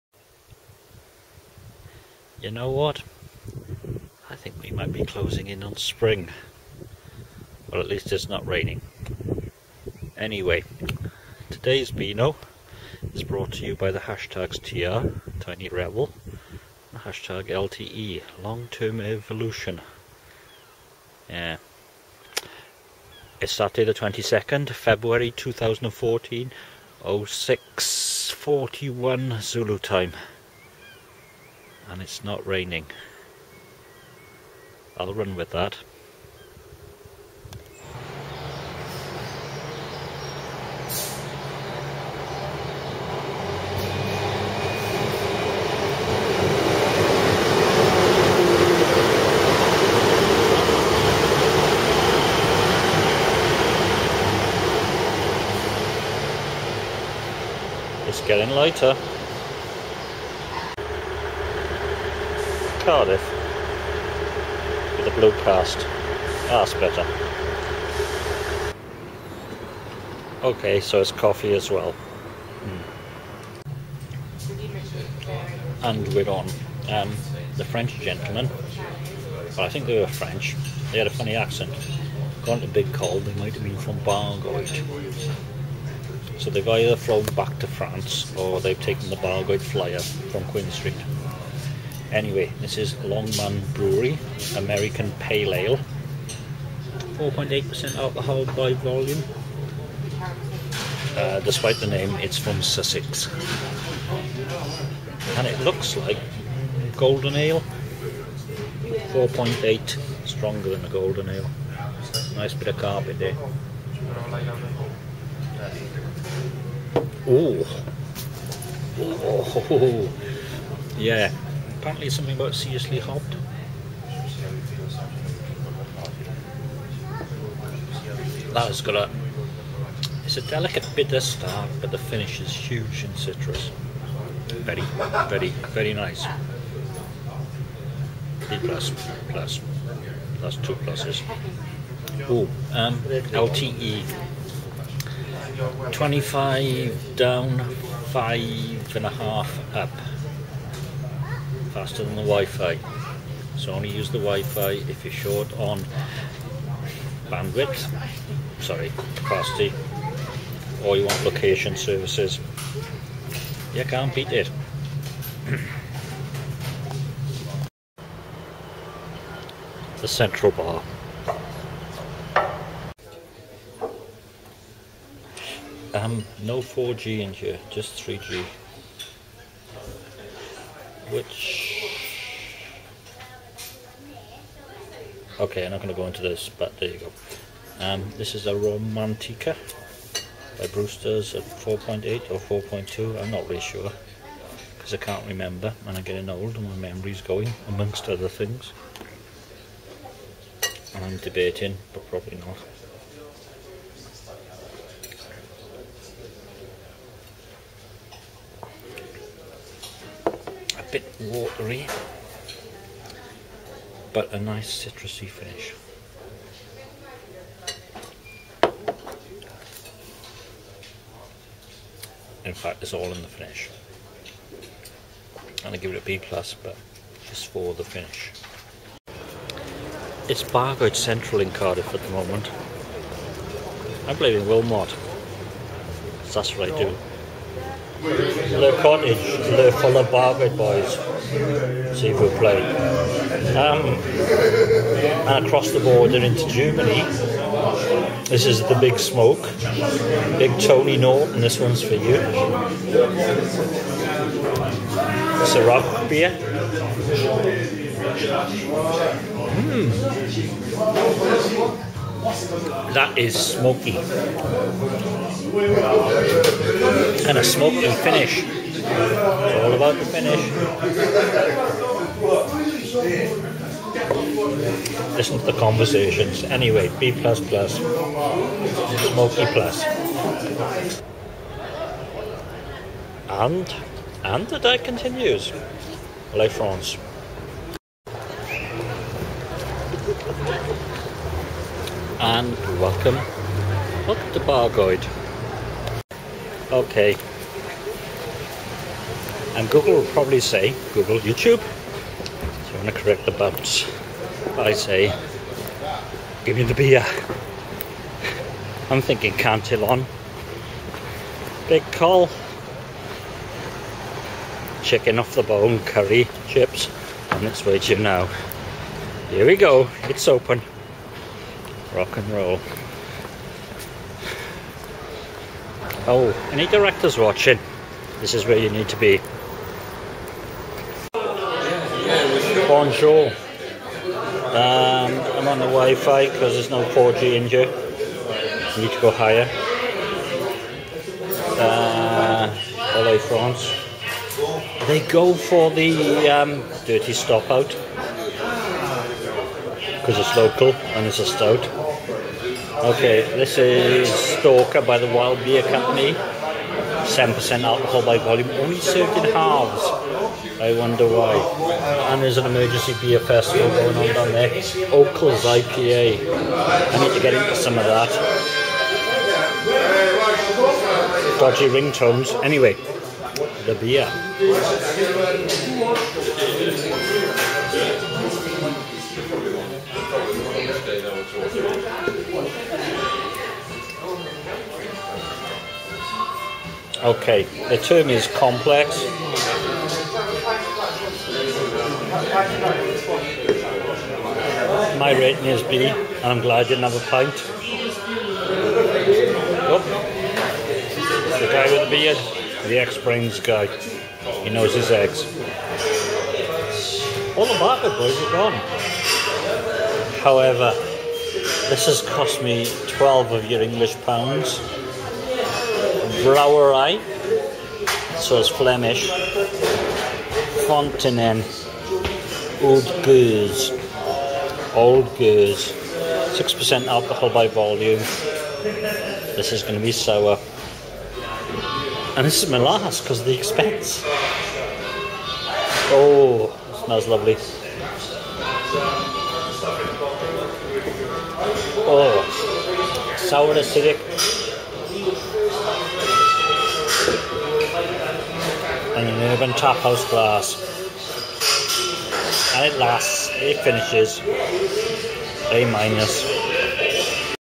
Bargoed or France
Cardiff on the day after the Welsh beat the French and everyone is talking funny.  In other news, I get LTE and a full day's battery life, yes a new phone.  Also in other, other news, the sun shines.